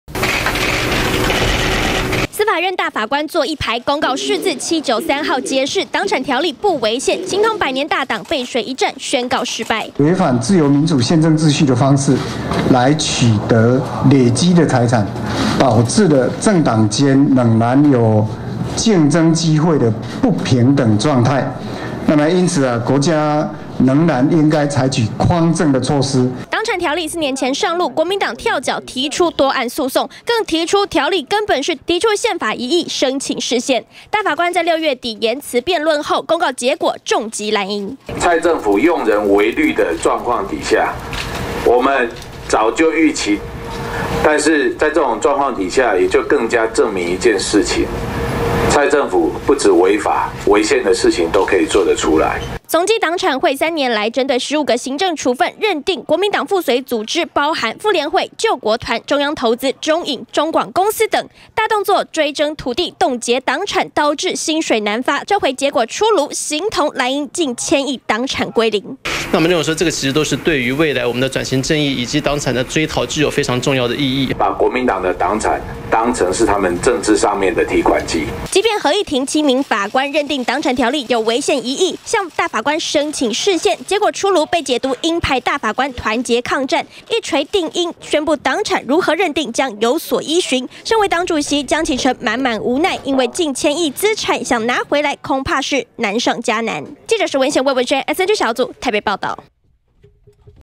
20200828中天新聞 黨產條例「全部合憲」！ 國民黨「千億」黨產歸零？
大法官一錘定音，國民黨千億資產保不住了，28號司法院大法官做出釋字第793條解釋，「黨產條例」全部合憲，國民黨慘遭輾壓，黨主席江啟臣表示結果不意外，因為司法院15位大法官中有11名是蔡總統提名任用，江啟臣說這證明一件事，蔡政府不只違法，連違憲的事情都做得出來，身為黨主席，江啟臣滿滿無奈，近千億資產想拿回來，恐怕難上加難。